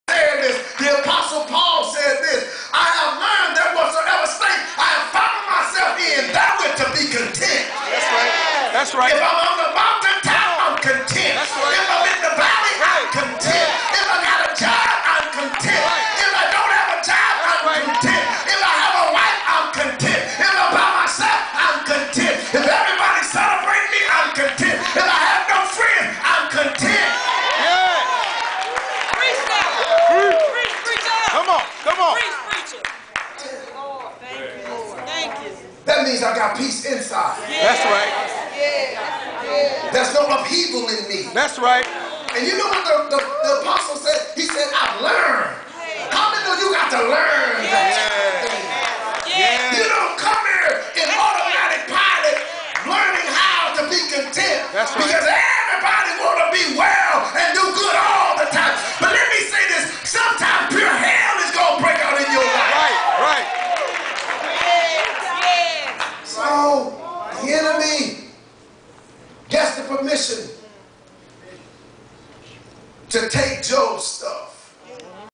Be Content in State You Are In 7 26 09 014
Bishop Larry Baylor sermon "Why Didn't You Tell Me"